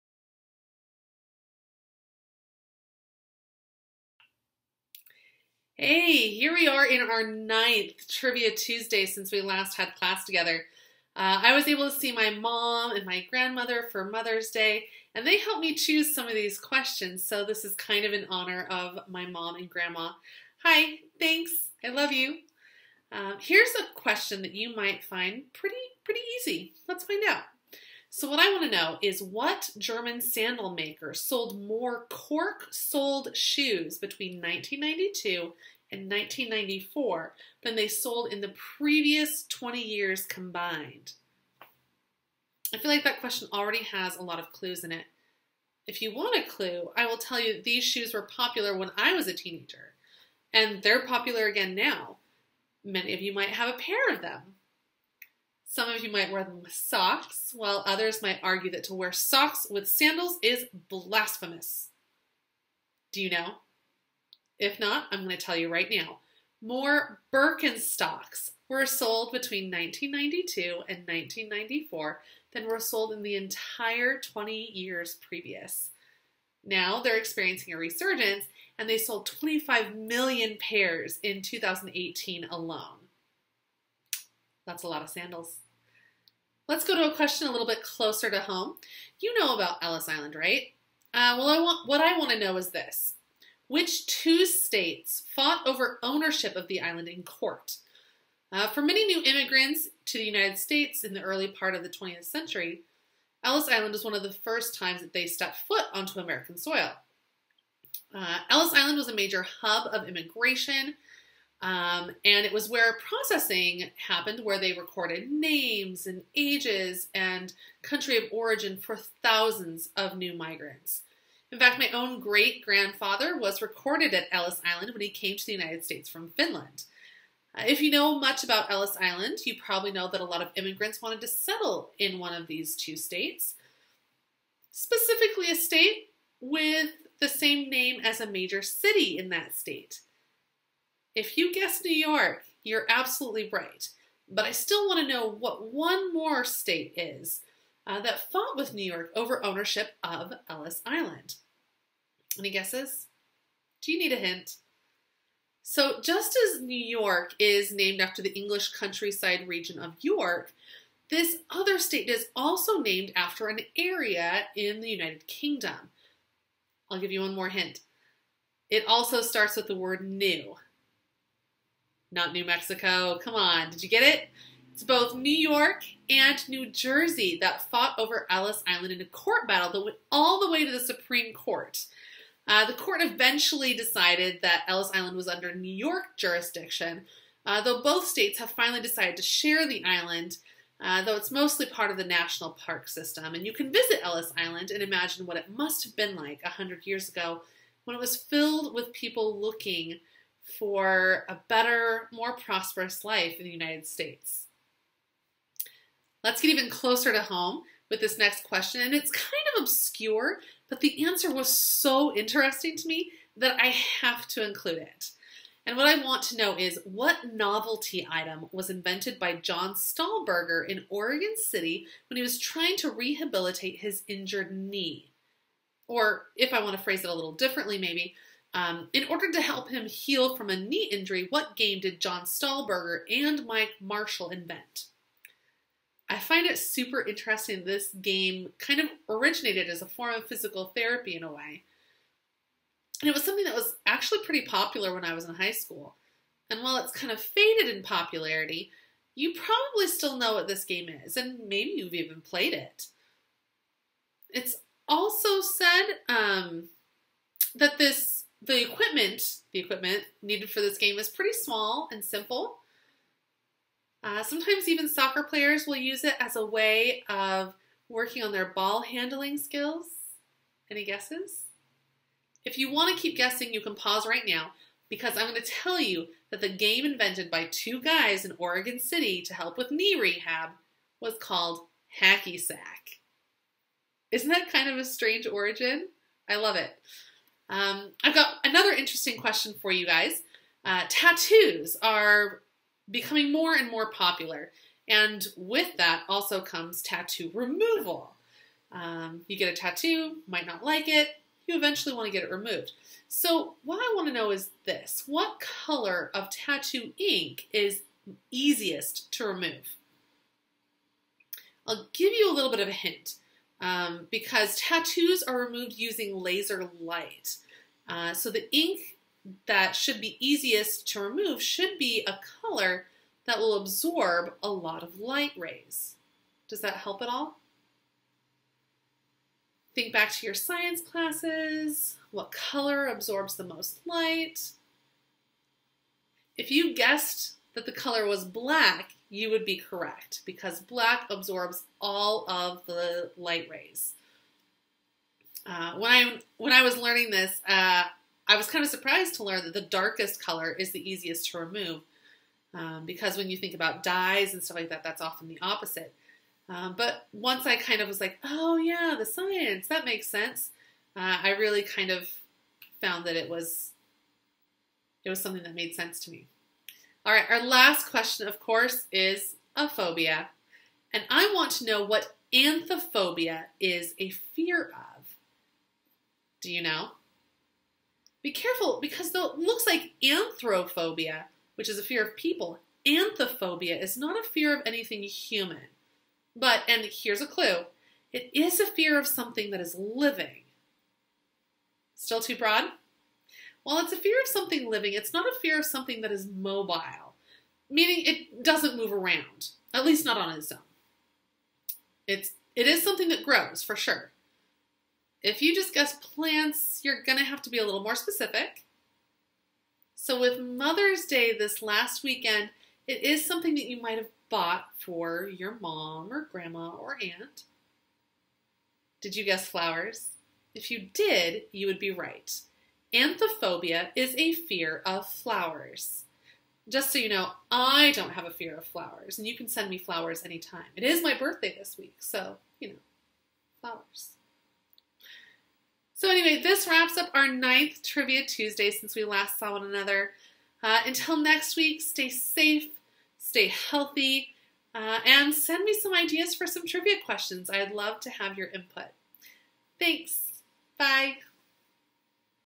Trivia Tuesday Week 9
It's our 9th week since we were last in school. Here's another week of socially distant Trivia Tuesday!